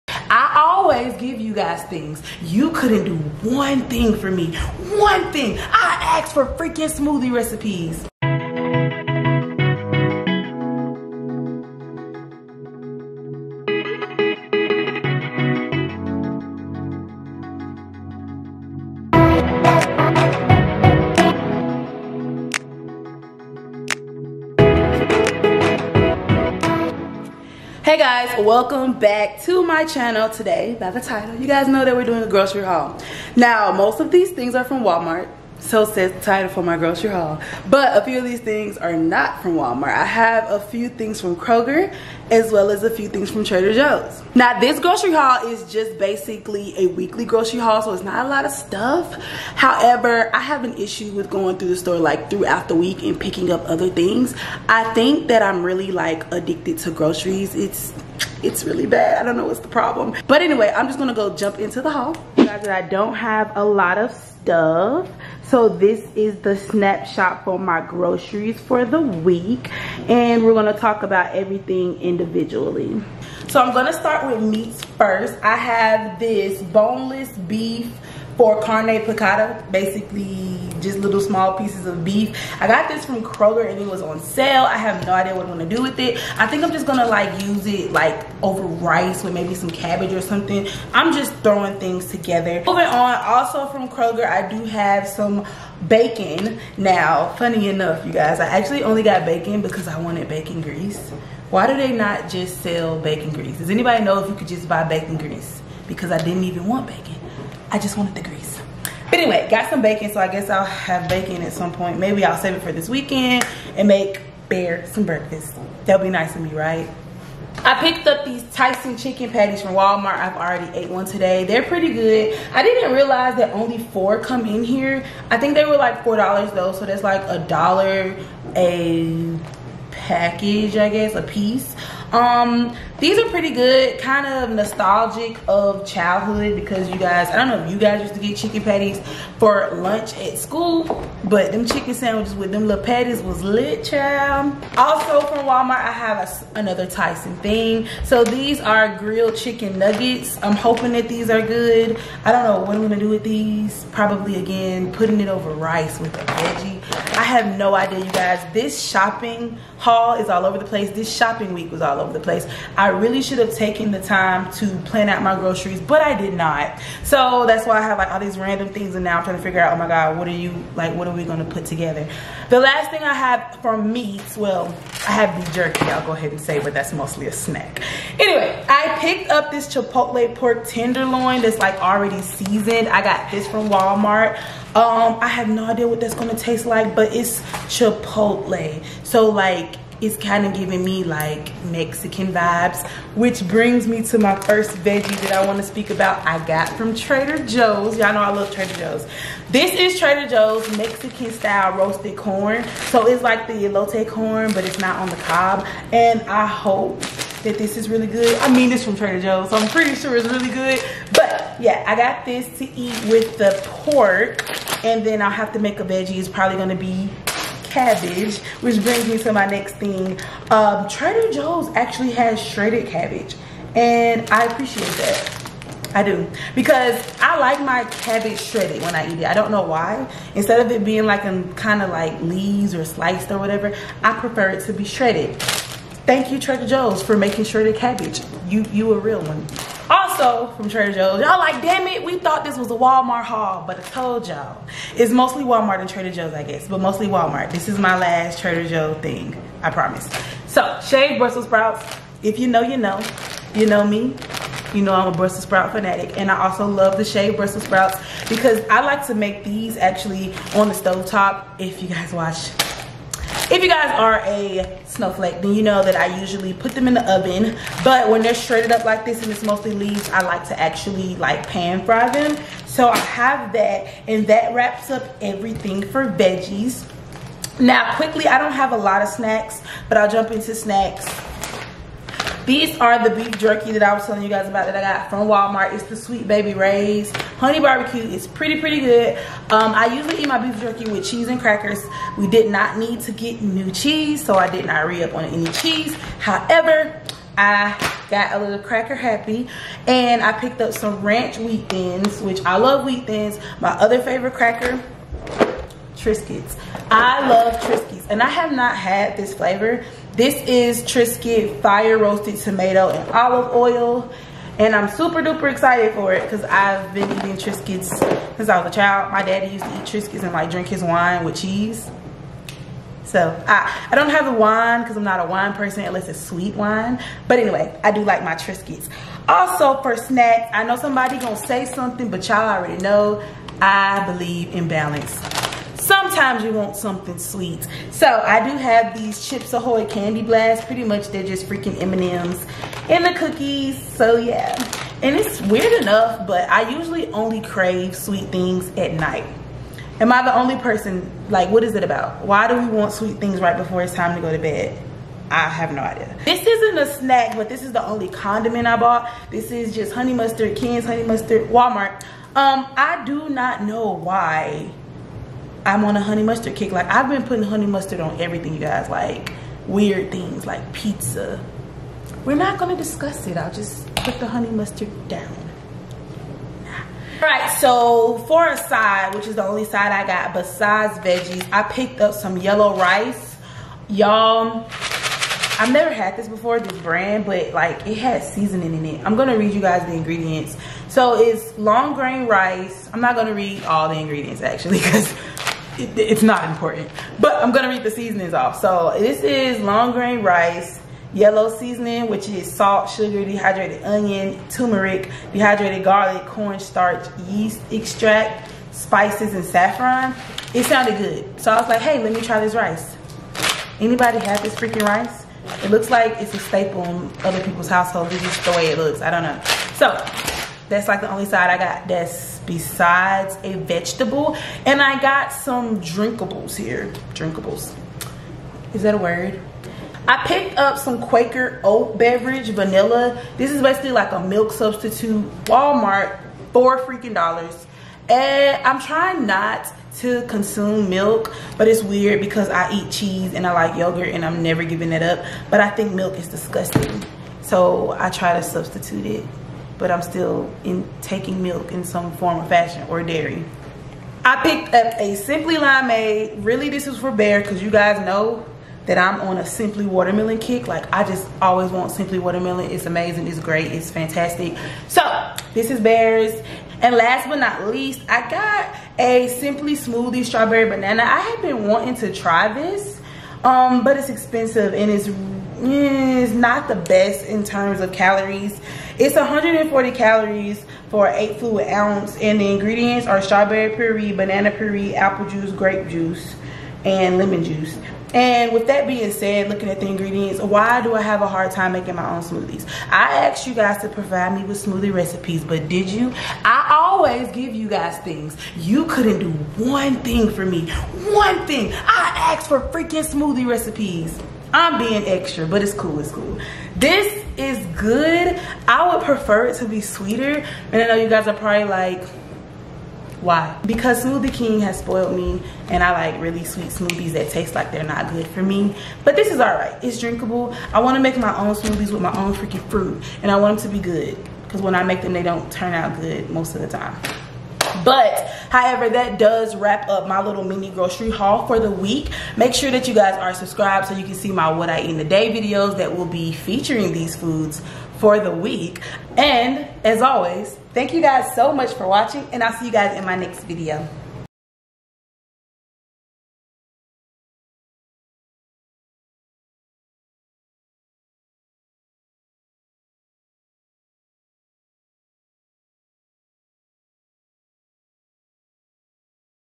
Weekly Grocery HAUL 2019 | Walmart, Kroger, & Trader Joes
My weekly grocery haul 2019! I picked up items from walmart, kroger, and trader joes!

Send me Love!
15 Franklin St. #687
Avondale Estates, GA 30002